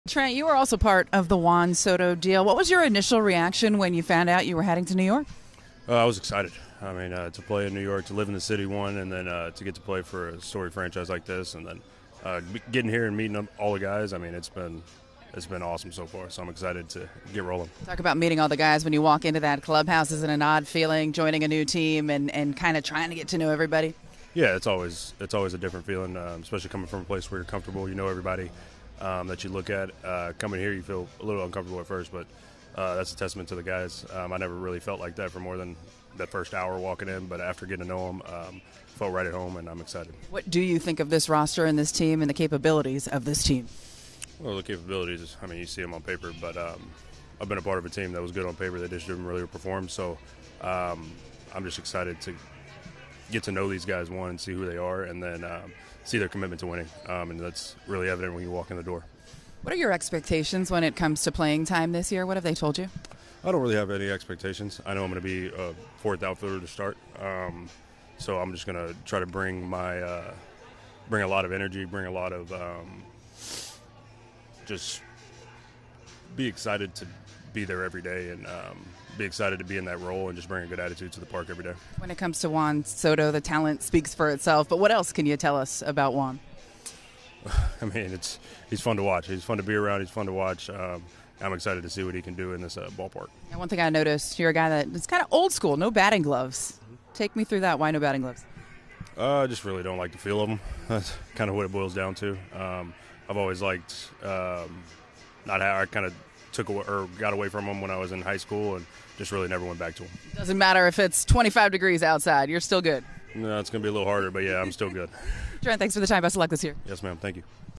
Trent Grisham embracing his role with 2024 Yankees
Trent Grisham joins Meredith Marakovits on the verge of his first season with the Yankees after coming over with Juan Soto from the Padres this offseason.